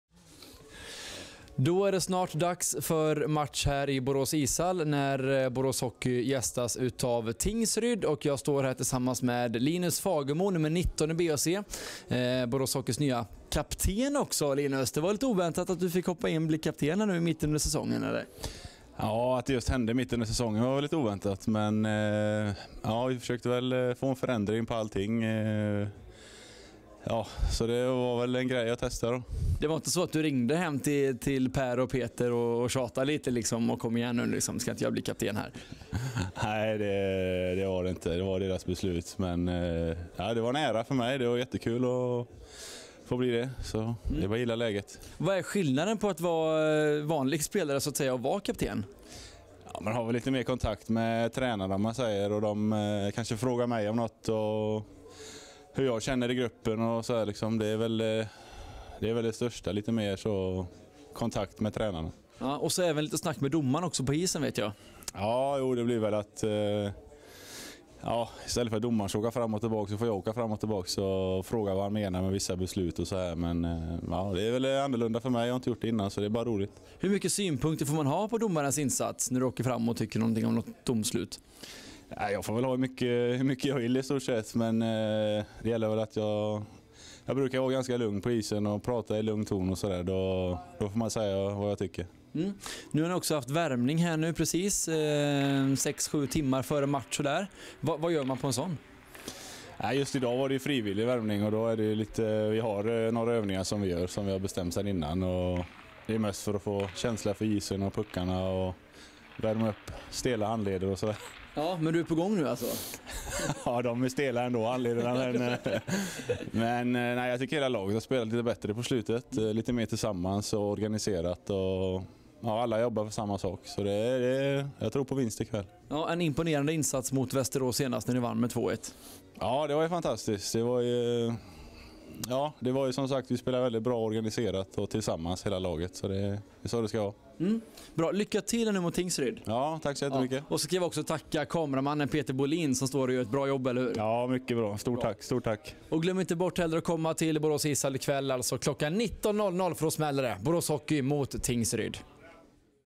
Inför Borås - Tingsryd
Linus Fagemo #19 intervjuas under värmningen inför Borås Hockey - Tingsryd 6/12-2011